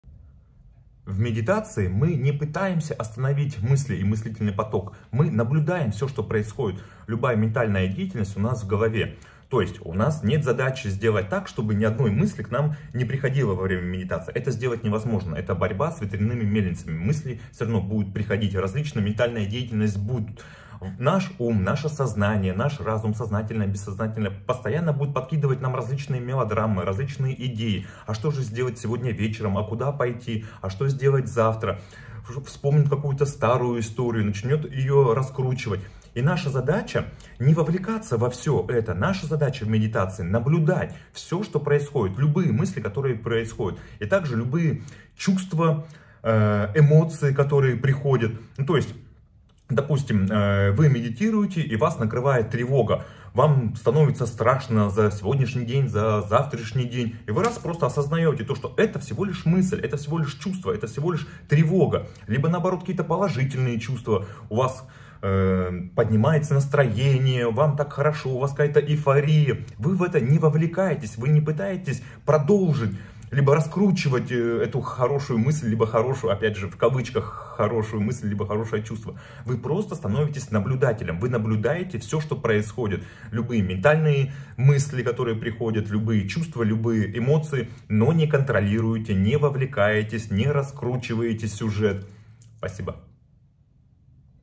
В медитации мы не останавливаем мыслительный поток, мы становимся наблюдателем
💙 Я помогу вам сделать первый шаг к медитации.

🙏 благодарю вас за комментарии под видео и ваши вопросы
🚀 всё это мотивирует создавать для вас новые видео

☎ Открыт для коллаборации, и сотрудничества